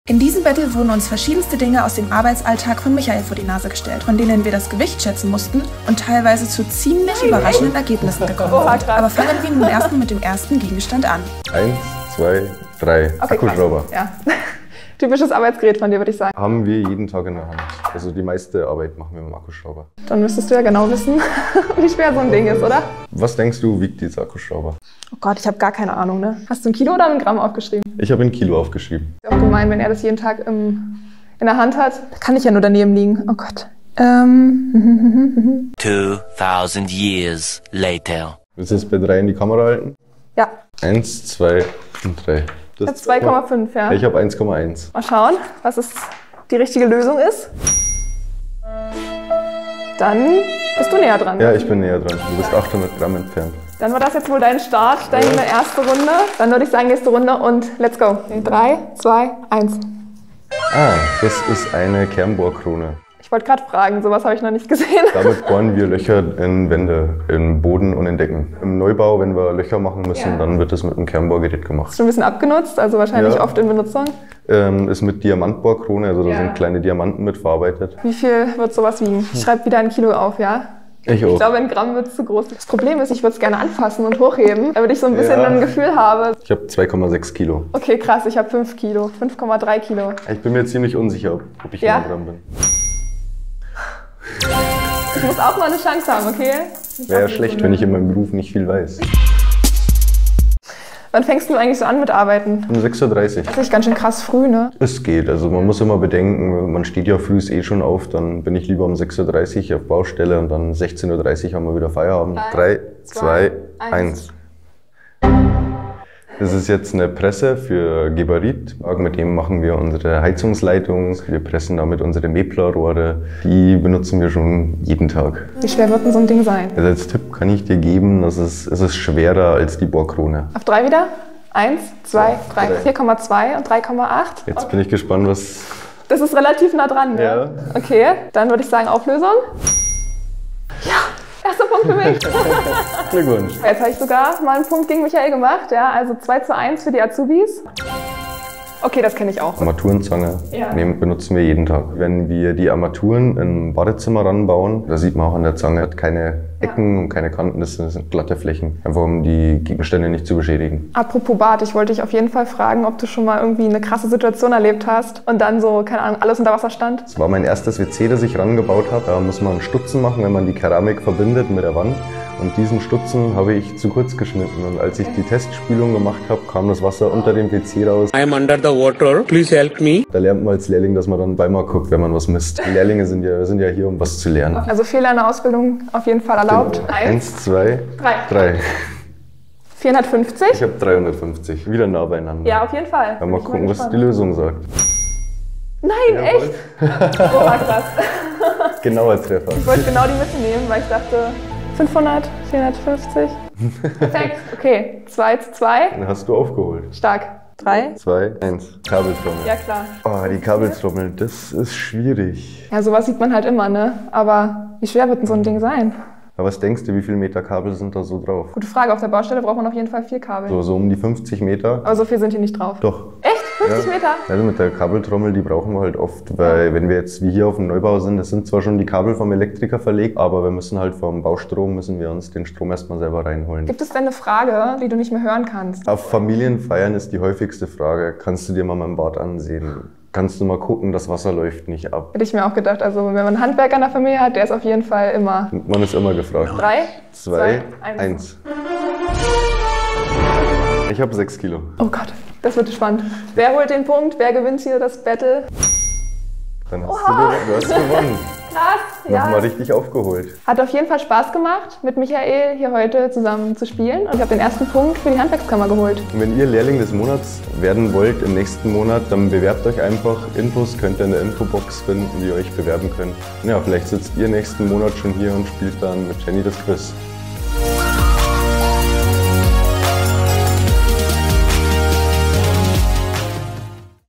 Anlagenmechaniker SHK💦🛠 im DUELL | #1 | Azubi vs.
✨In diesem spannenden Video tritt Michael (Lehrling des Monats - Februar 2024, Ausbildungsberuf: Anlagenmechaniker SHK), gegen Jenny von der Handwerkskammer Cottbus in einem aufregenden Battle an.

🦾Die beiden müssen das Gewicht typischer Arbeitsmittel eines Anlagenmechaniker SHK schätzen und in 5 spannenden Runden gegeneinander antreten.

🏆Wer wird am Ende als Sieger hervorgehen? Finde es heraus und erlebe mit, wie die beiden Kontrahenten um den Sieg kämpfen! Ein Video für alle Fans vom Handwerk und Wettbewerben.

KAPITEL_____________________________________________
00:00  Intro
00:11  Runde 1: Akkuschrauber
00:58  Runde 2: Kernbohrkrone
01:52  Runde 3: Geberit Presse
02:27  Runde 4: Armaturenzange
03:27  Runde 5: Kabeltrommel
04:20  Outro

Ihr wollt einen Punkt für das Team azubi holen: Dann bewerbt Euch jetzt für den Lehrling des Monats!